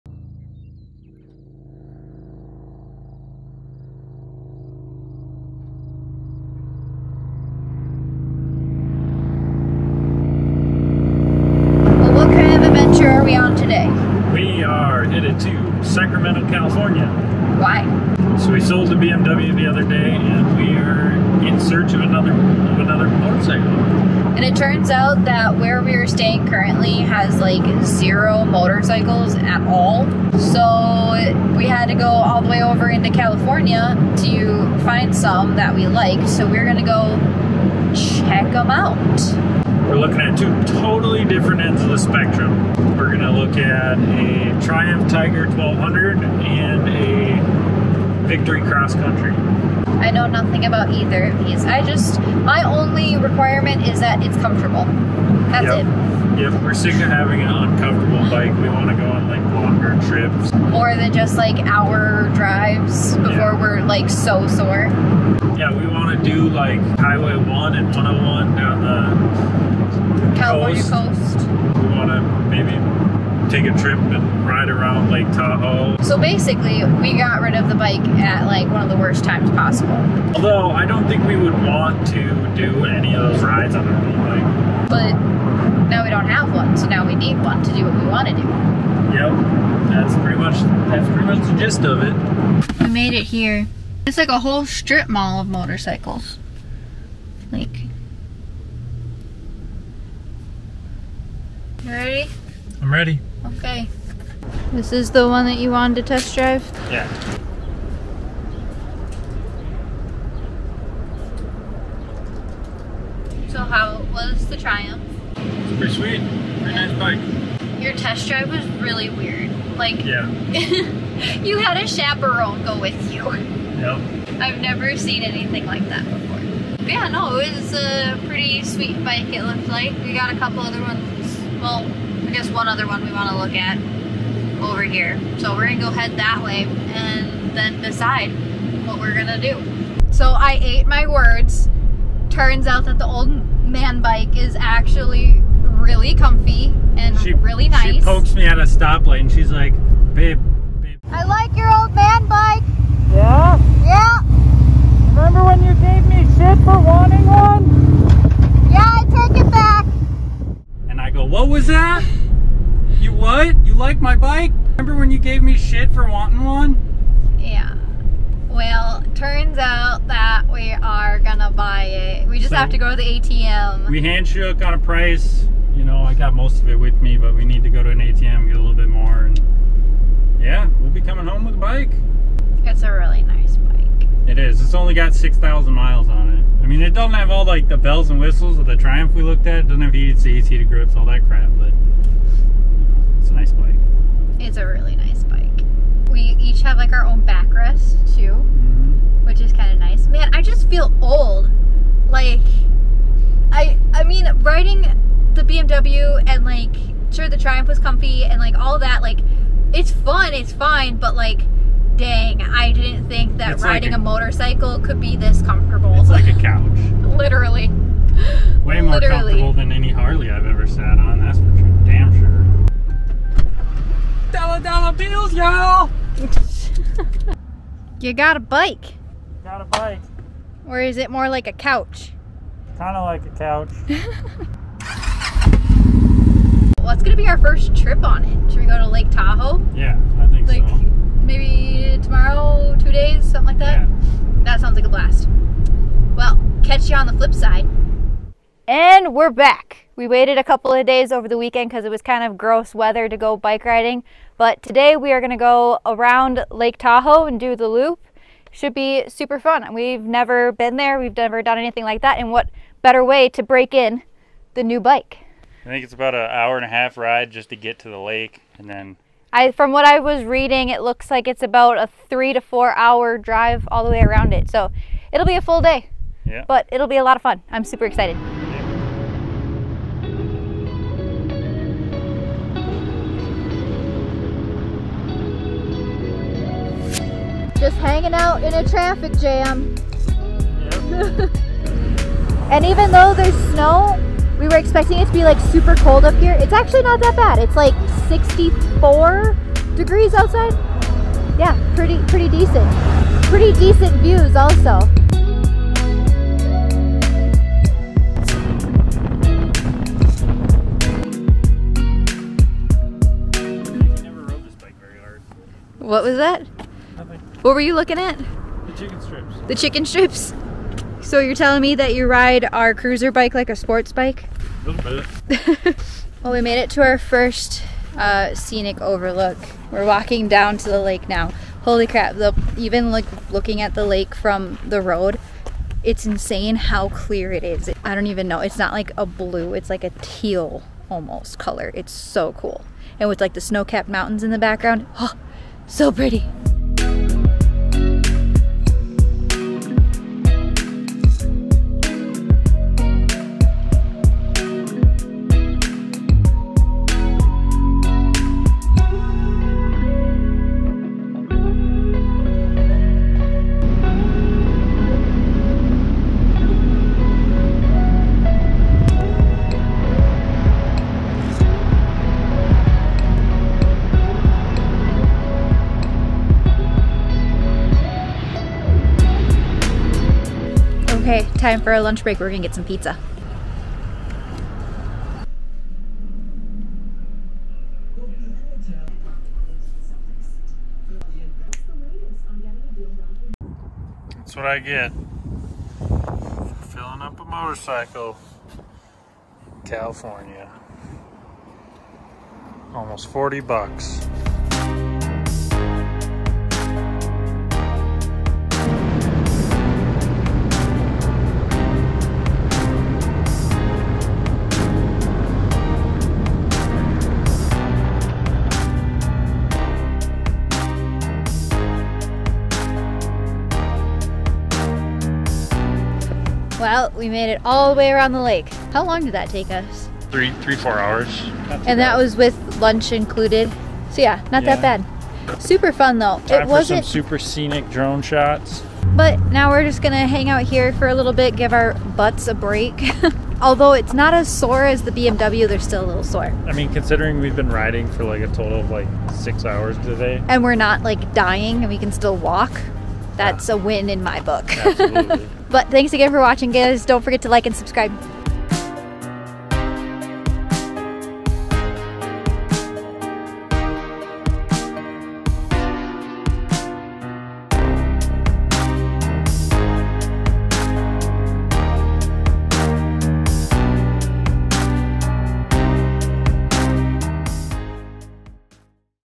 How Many Vehicles can you Bring Full Time RV Life
We sold the BMW R1200R motorcycle and had to get a replacement! Did we decide on the Triumph Tiger 1200 or the Victory Cross Country?

⛽💲⛽💲Get $10 in FREE FUEL! Using the Mudflap App⛽💲⛽💲

⛽💲Cash Back on Gas and Diesel through GetUpside!